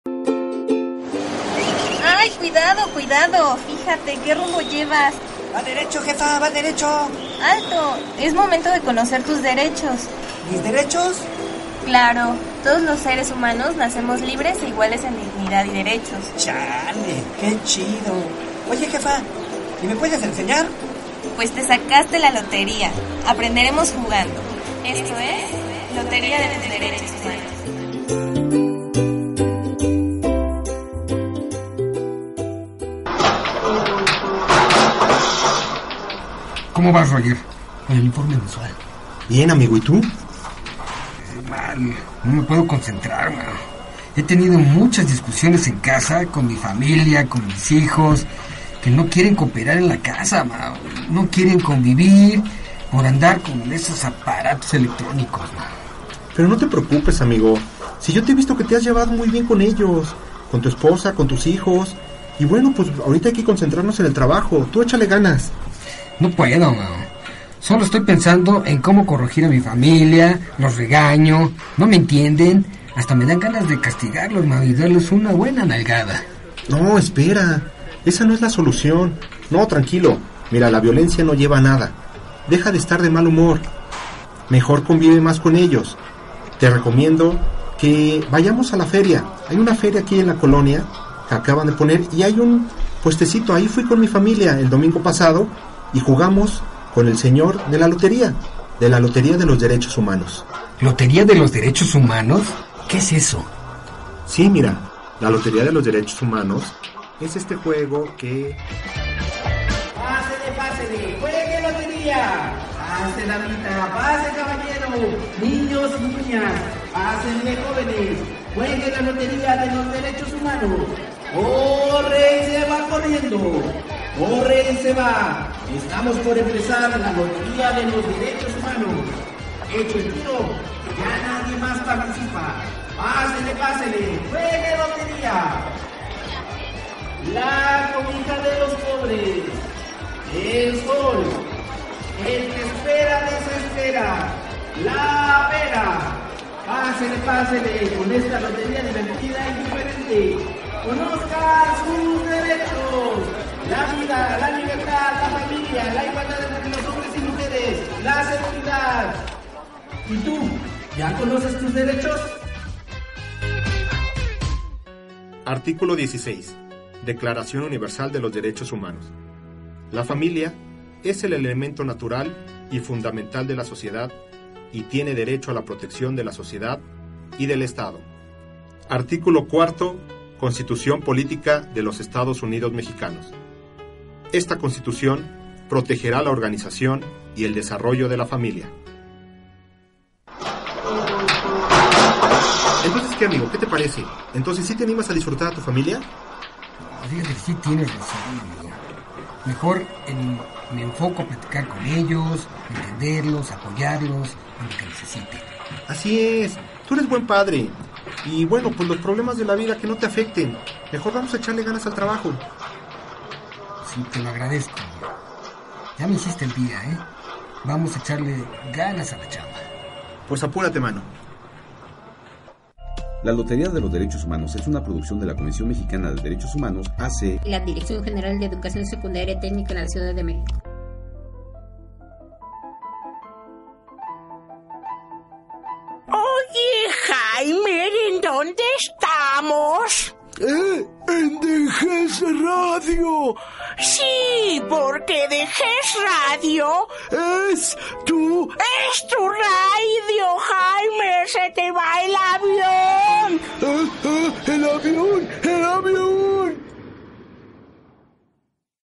CE 17-18 La lotería de los Derechos Humanos. "Derecho a la familia"
El derecho familiar es el conjunto de normas e instituciones jurídicas que regulan las relaciones personales y patrimoniales de los miembros que integran la familia, entre sí y respecto de terceros. Descárgalo en mp3